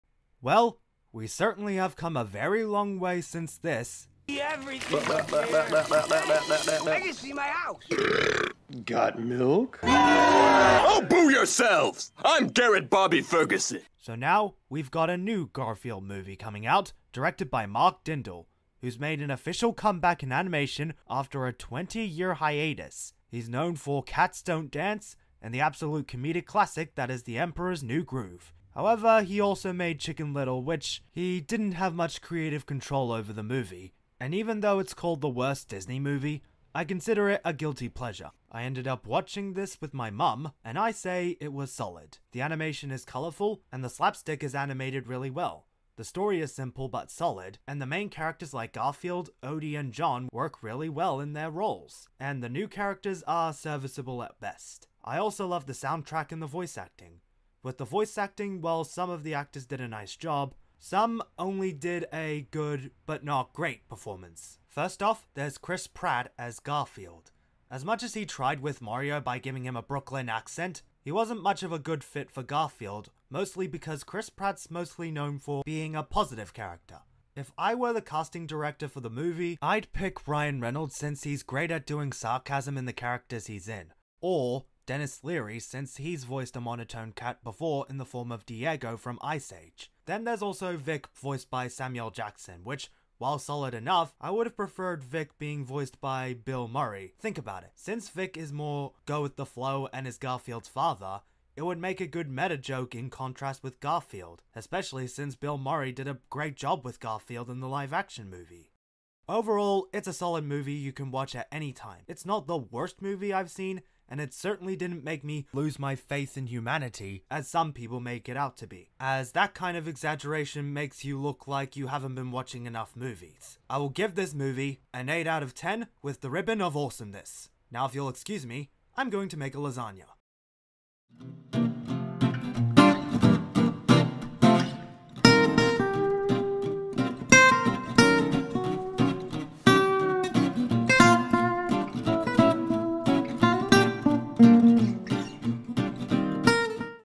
The Garfield Movie is pretty decent!
I have absolutely no idea why I'm reviewing this on Monday. Well it is a quick thoughts video on Garfield so.